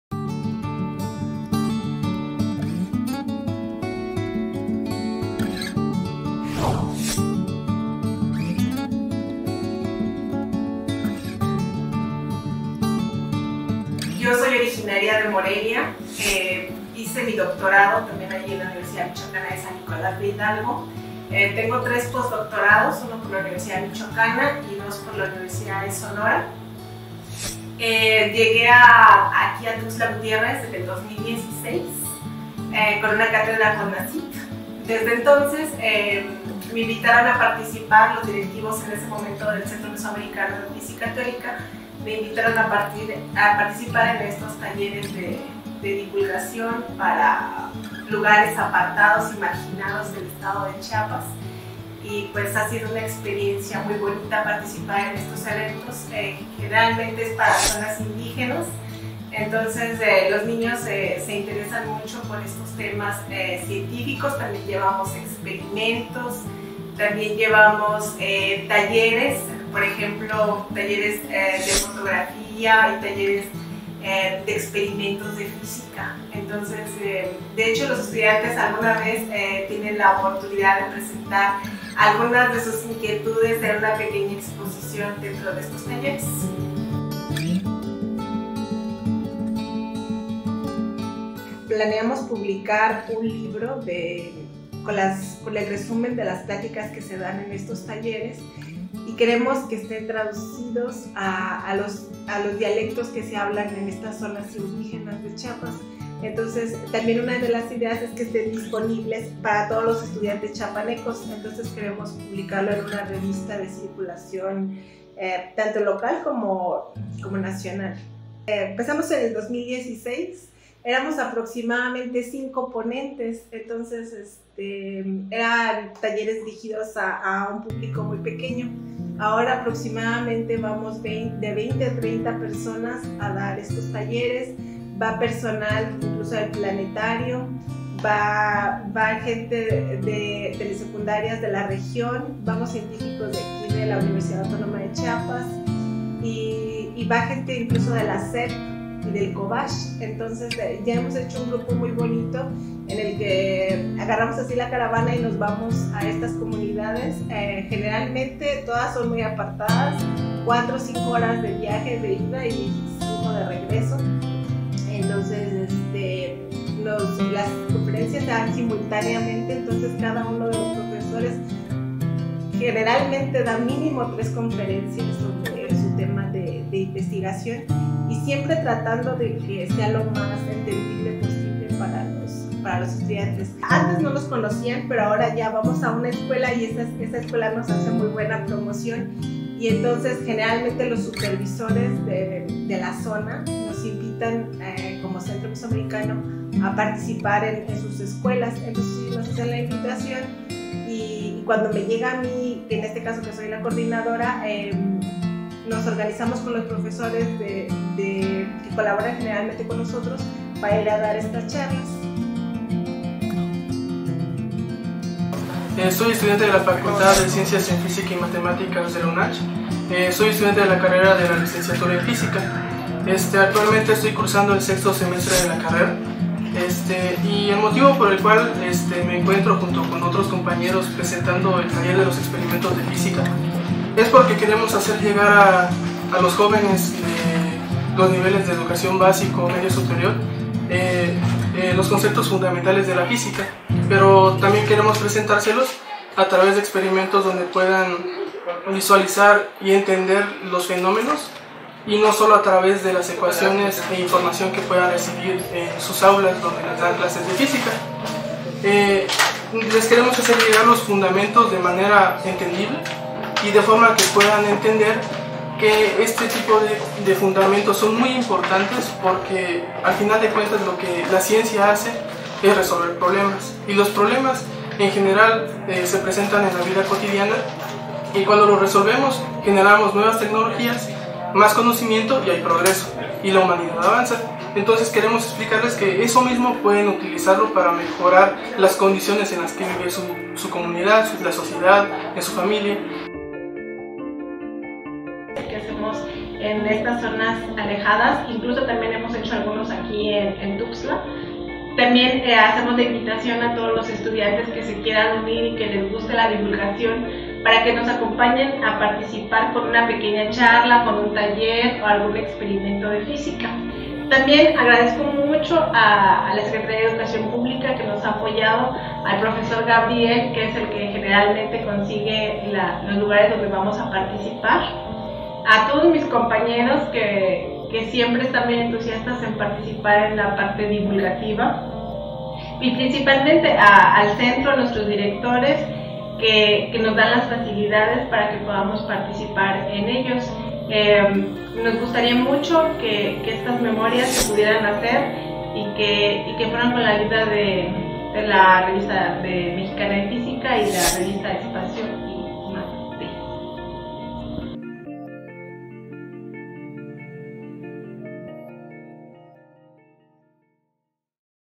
Talleres de Ciencia y Tecnología del MCTP.
El MCTP se compromete a fomentar la educación científica regional desde los niveles básicos del sistema educativo. Con el fin de cumplir con este objetivo el MCTP se coordina con profesores de Universidades de la región mesoamericana y de los planteles de nivel medio y medio superior para organizar el VII Ciclo de Talleres en Ciencia y Tecnología 2022.